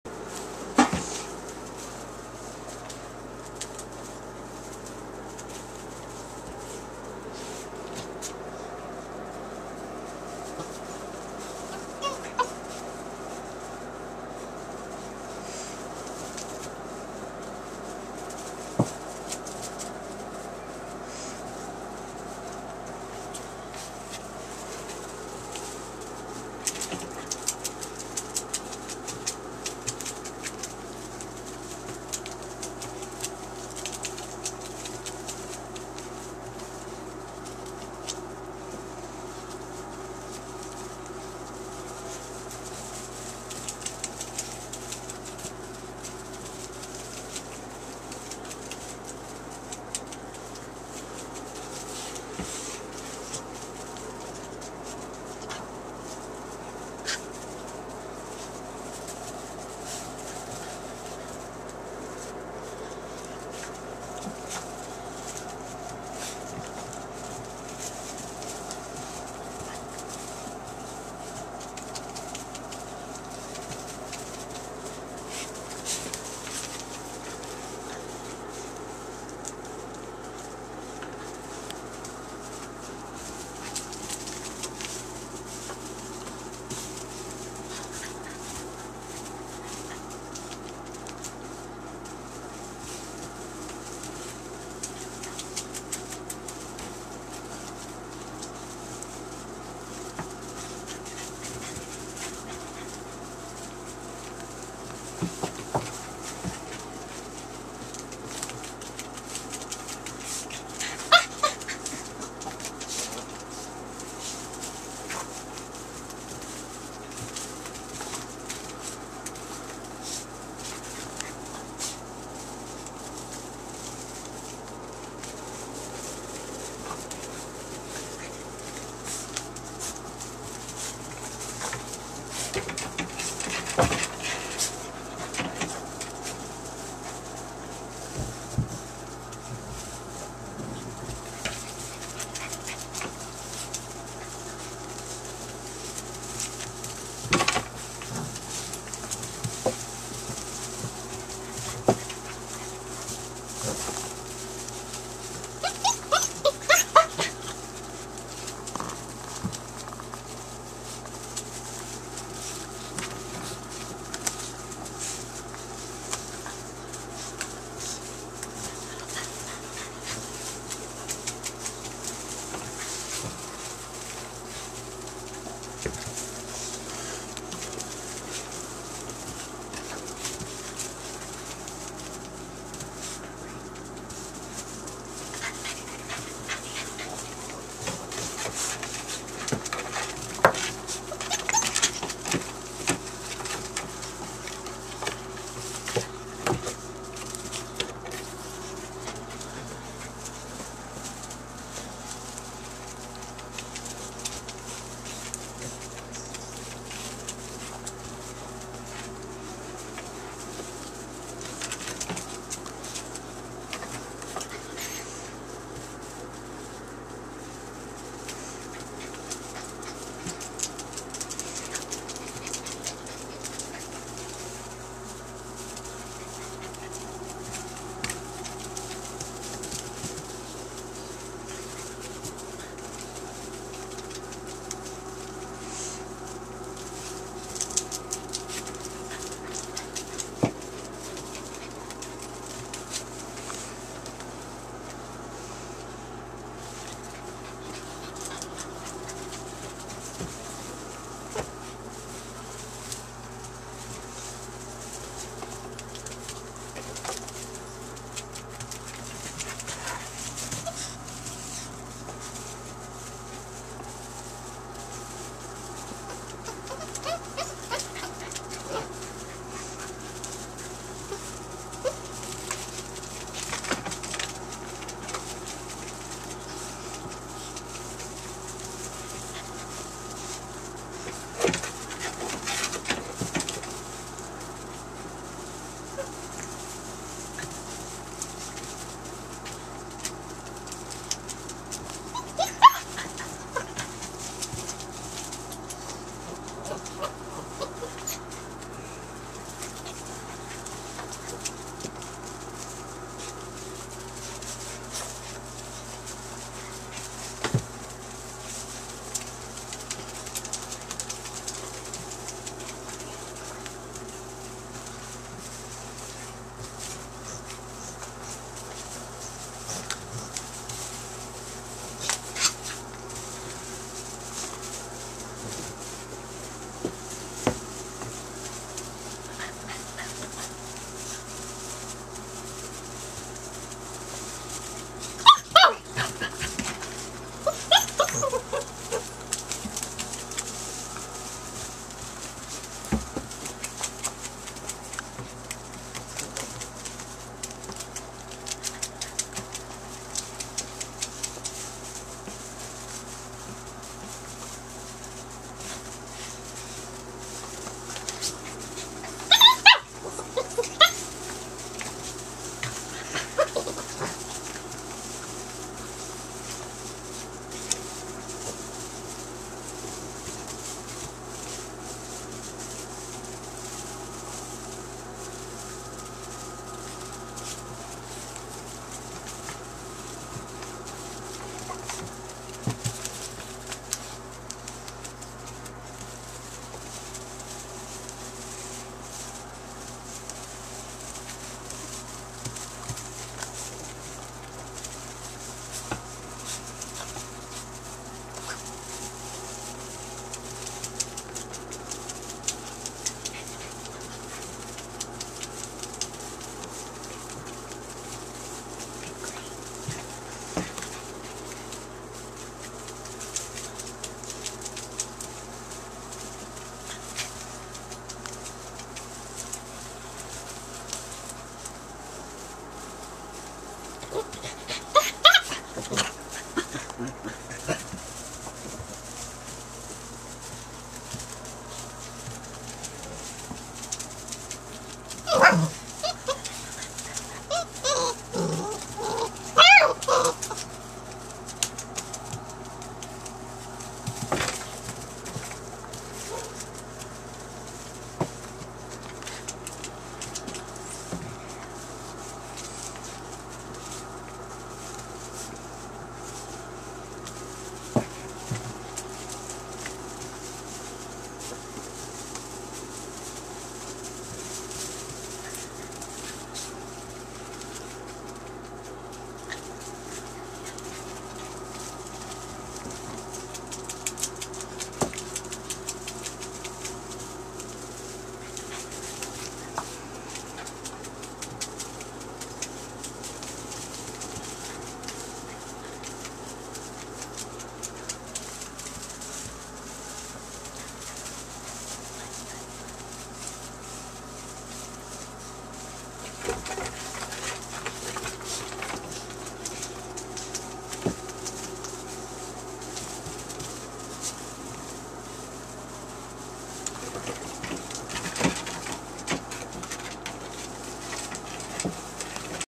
Cougar x Fire puppies 5 1/2 weeks
Boys are Red , Green, Blue and Girls are White, pink, Purple.